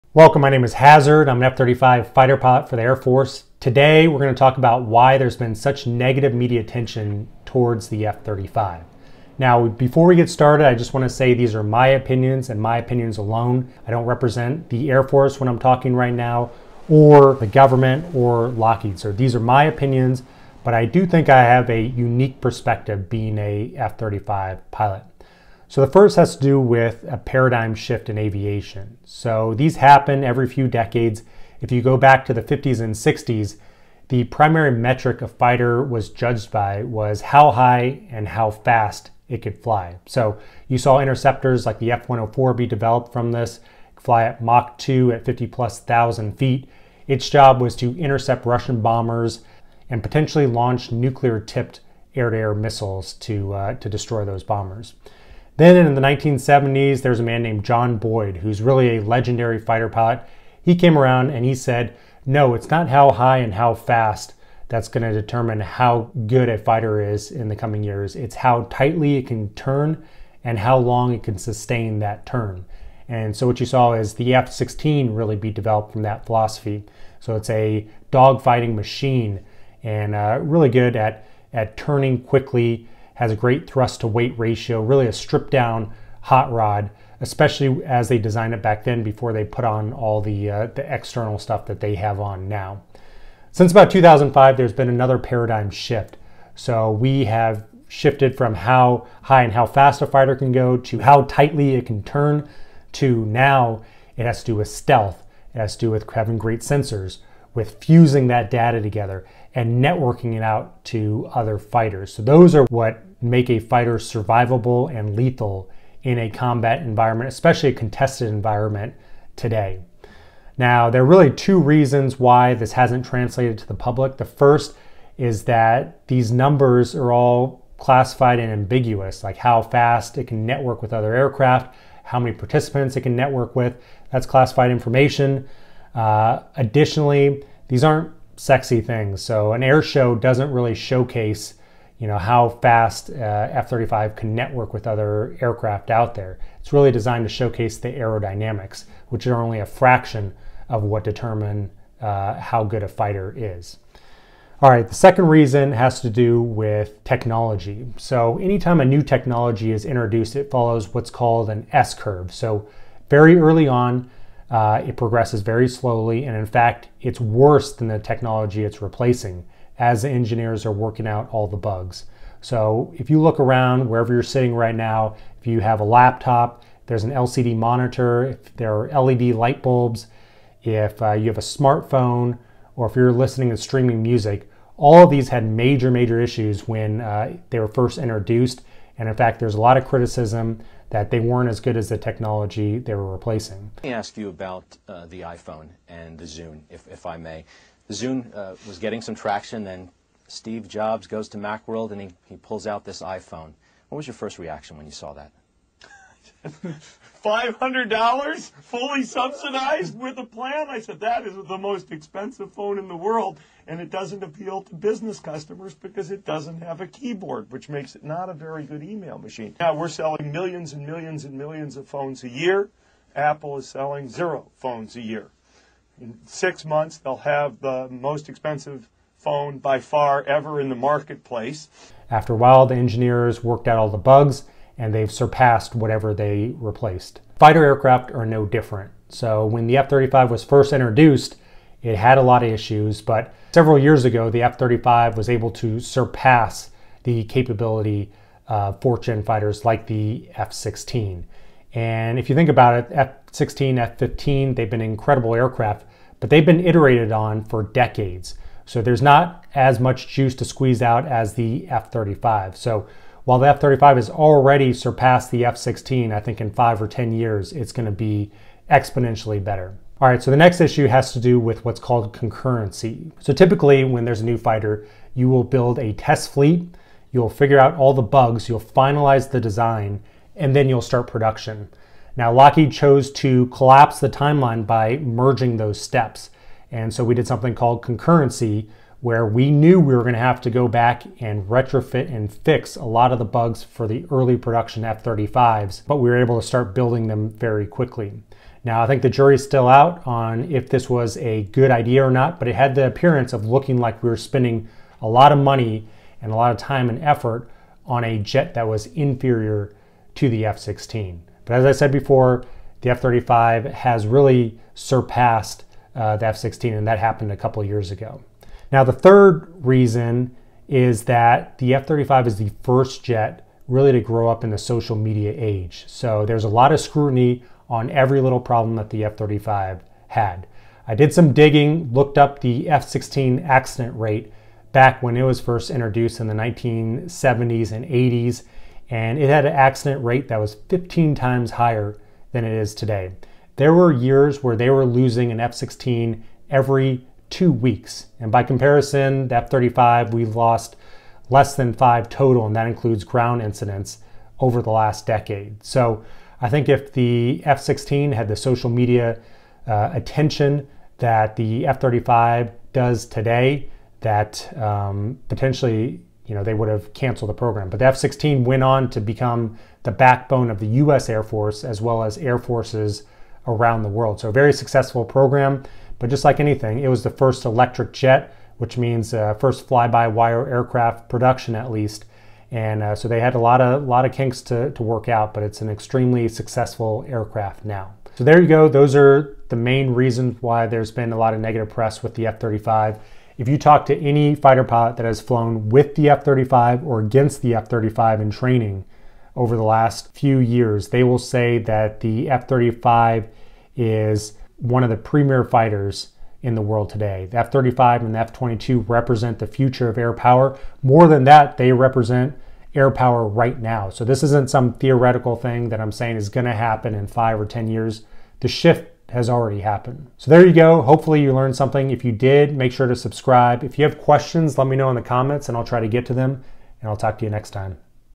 WHY has the F-35 received so much Negative Attention? Real Fighter Pilot Explains
A video on why the F-35 has had so much negative press by Fighter Pilot Hasard Lee.